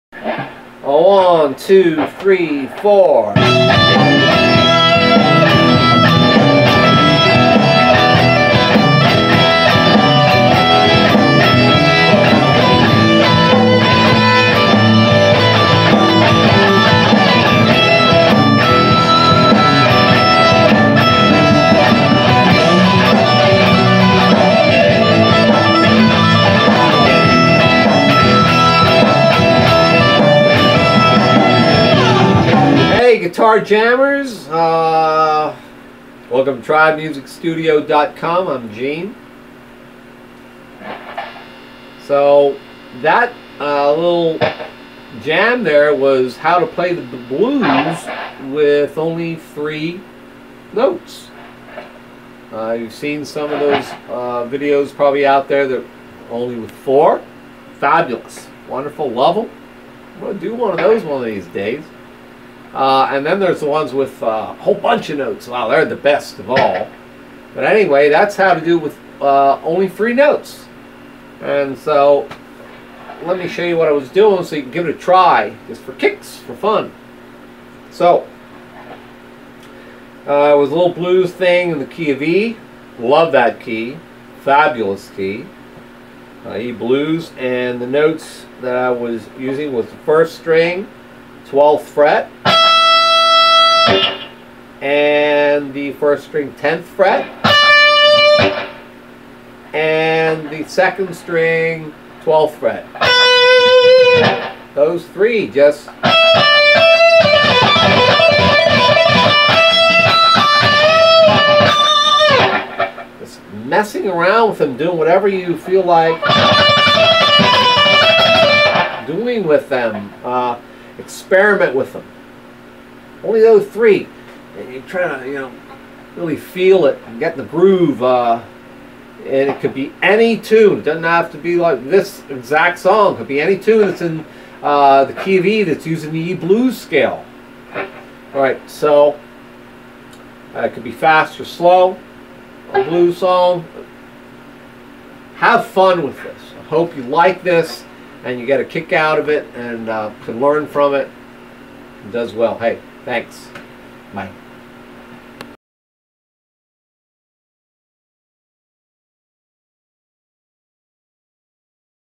How to Play this INCREDIBLY EASY 3 notes only Guitar Jam/Solo - L85
This is a Blues Jam in E where I'm using 3 notes only to jam along.
This is my arrangement of this piece.